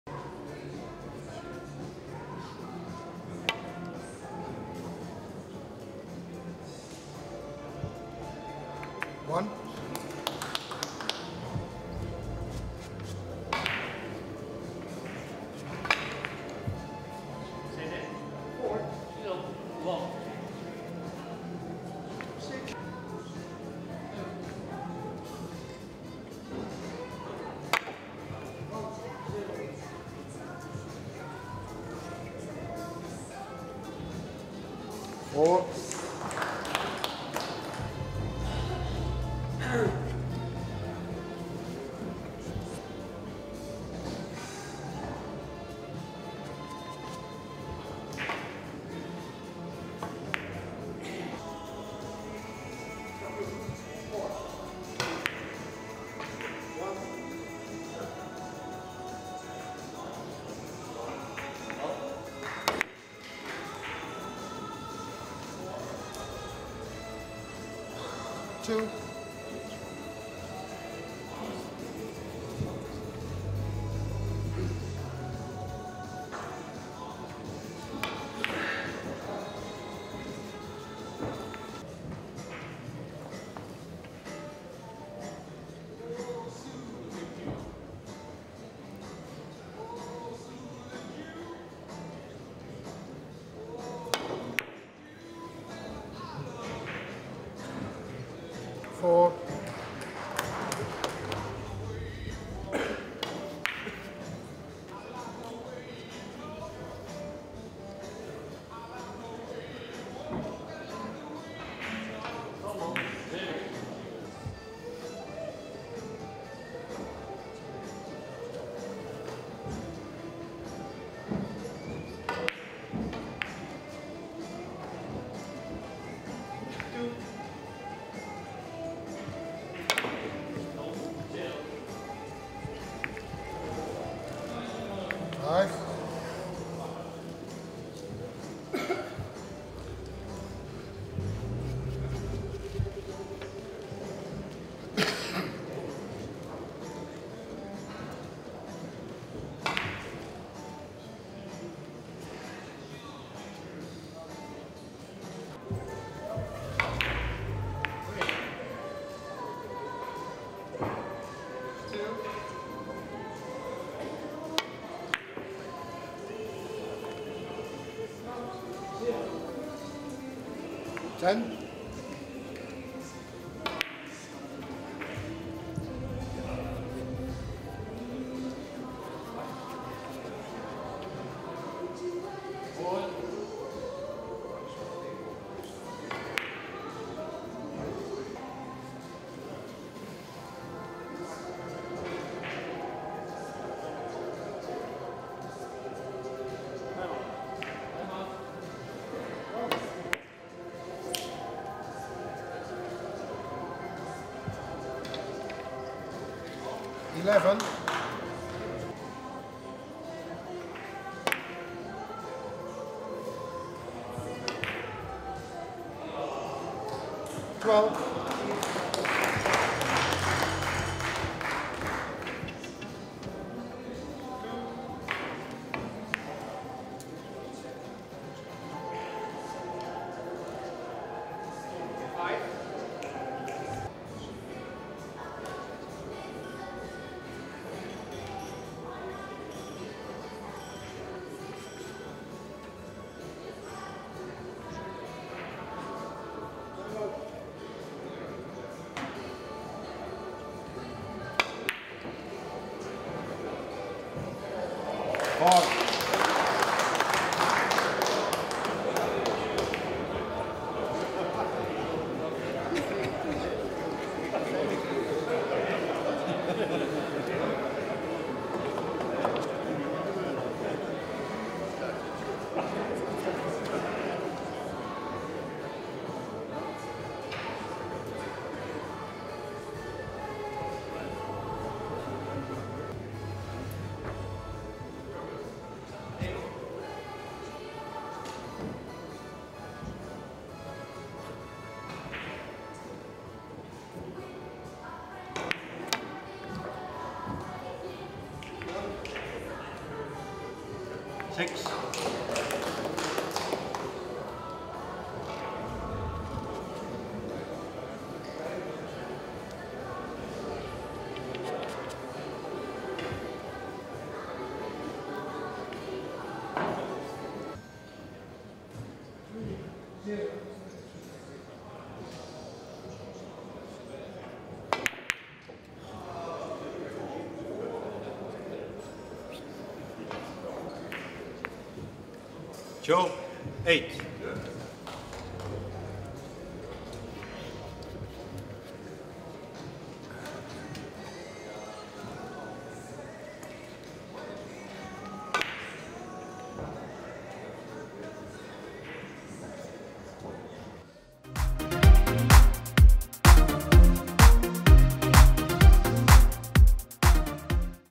[2019 베겔 3쿠션 월드컵] 마틴 혼 vs 조명우 하이라이트 '방심은 금지입니다. 상대는 조명우에요'
코줌 인터내셔널 유튜브 채널